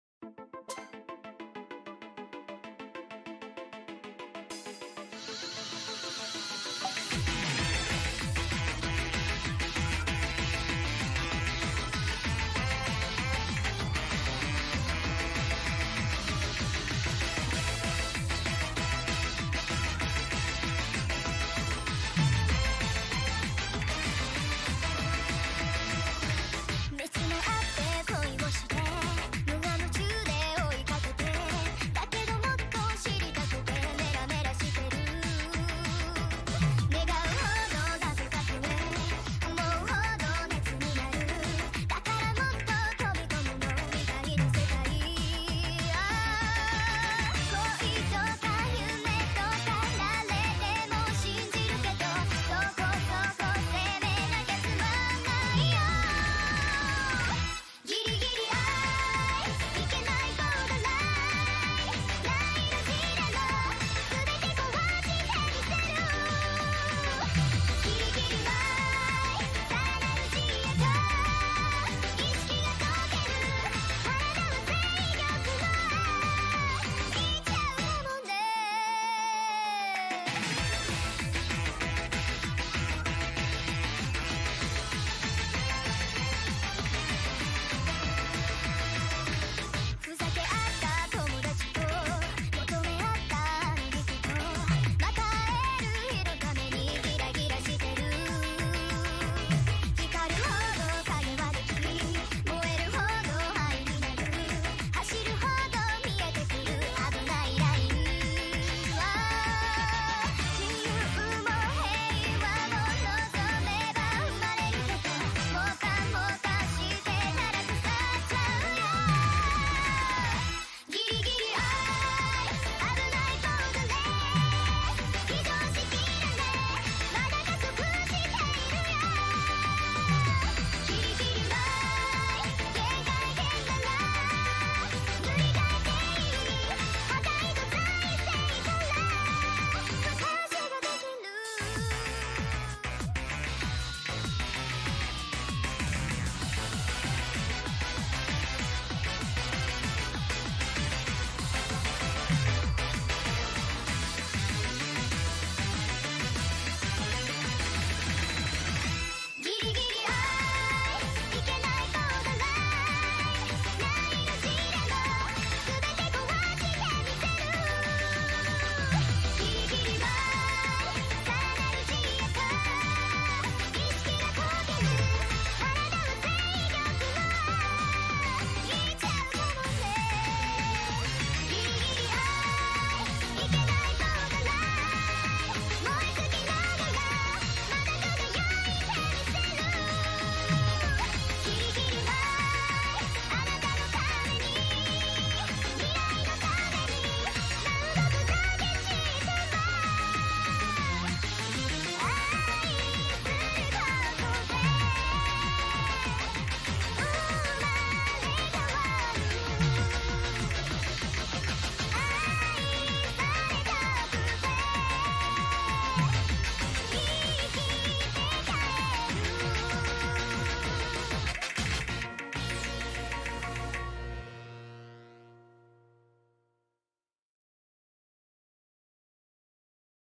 The offline Improvement arc begins (breaking my pp addiction)
great start

Post mortem: pp addict goes offline to break pp addiction. While offline I just pushed skill cap both aim and stamina.